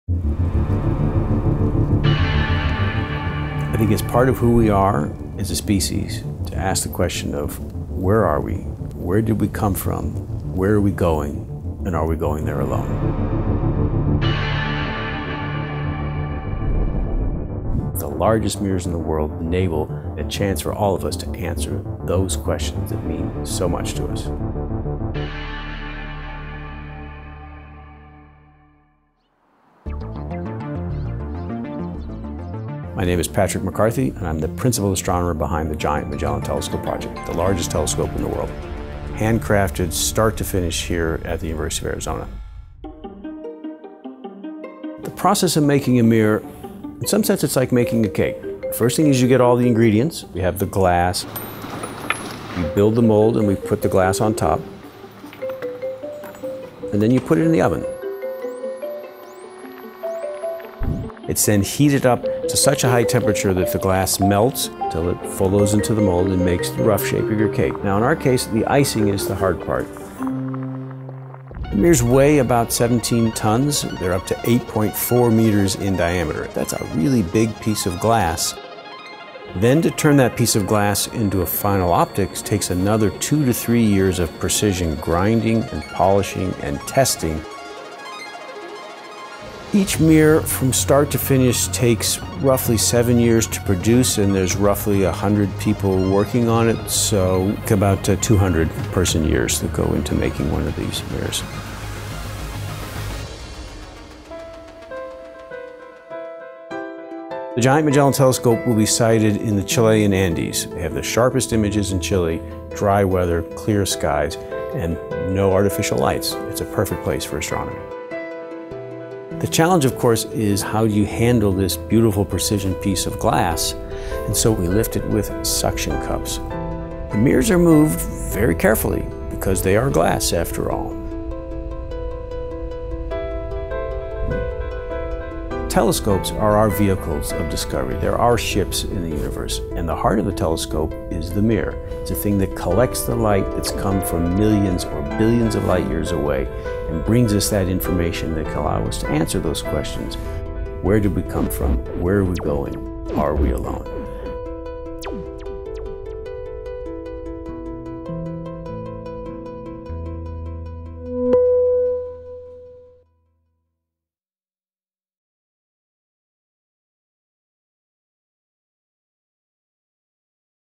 Building the World’s Largest Telescope
Underneath the University of Arizona’s football stadium are the beginnings of the Giant Magellan Telescope, set to be the largest optical telescope in the world. The Richard F. Caris Mirror Laboratory is the only place equipped to manufacture gigantic 27-foot tall mirrors, the kind needed to build the heart of the Magellan. Each of the seven mirrors that will comprise the telescope weigh in at 17-tons and will produce images with 10 times the resolution of the Hubble Space Telescope, giving scientists an unprecedented look into the fringes of our universe.